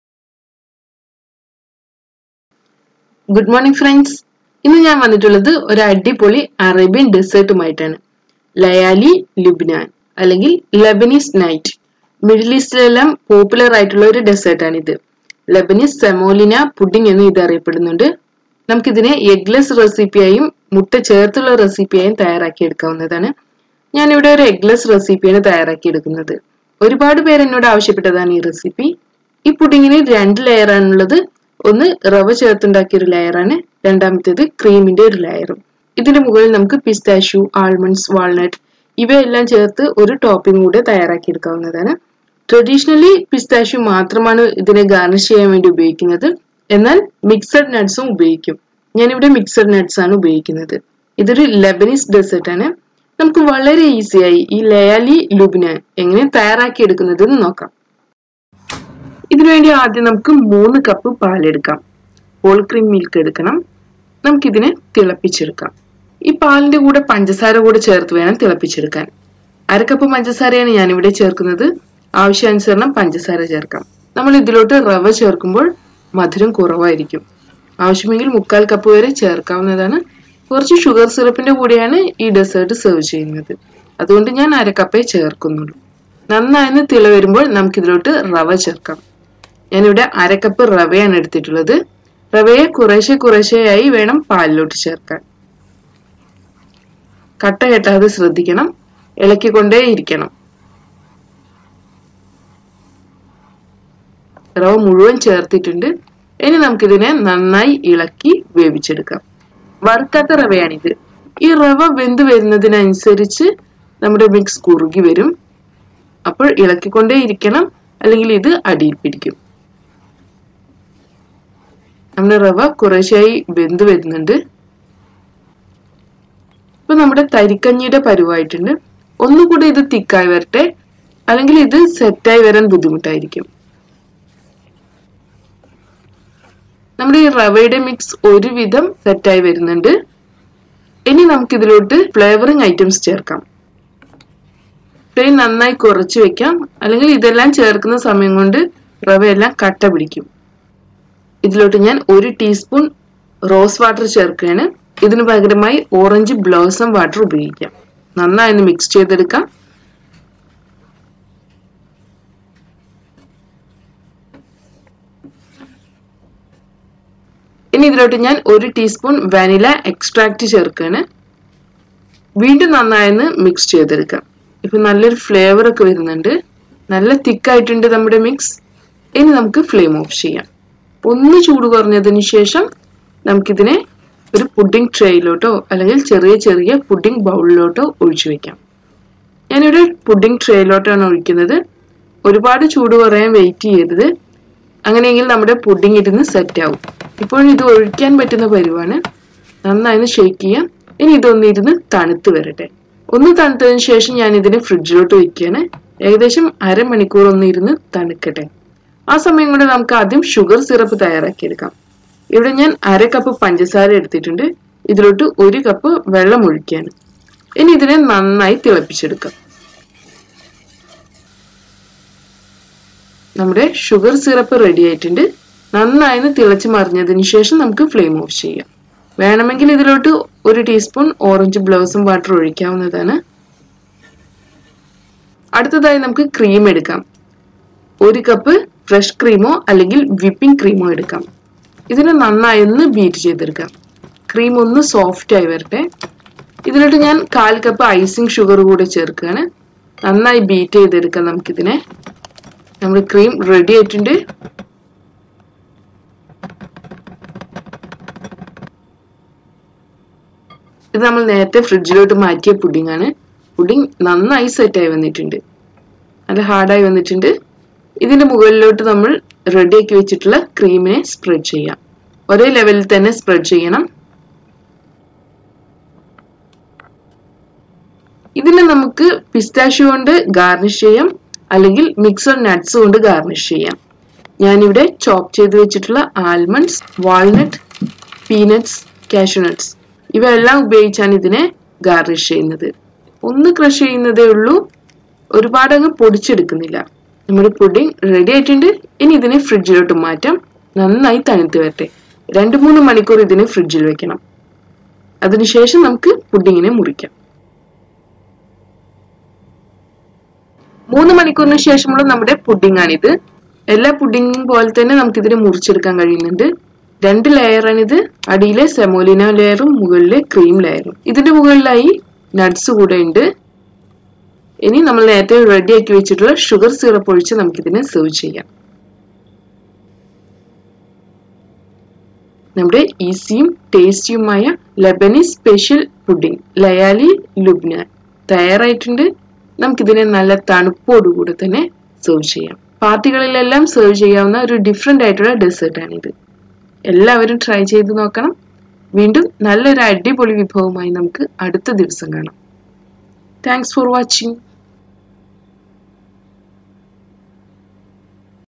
ലയാലി ലുബ്നാൻ Layali Libnan / Lebanese Nights Arabian Semolina Pudding
This video explain how to make easy and delicious Arabian Snack  'Layali Libnan / Lebanese Nights'. It is a delicious Arabian Semolina Pudding.

Layali Libnan / Lebanese Nights Arabian Semolina Pudding
Ingredients:
Rose water / Orange Blossom water – 1 Tsp.
Vanilla extract – 1 Tsp.
Fresh Cream / Whipping Cream – 1 Cup.
Chopped nuts like Cashew nuts, almonds, walnuts, peanuts  etc.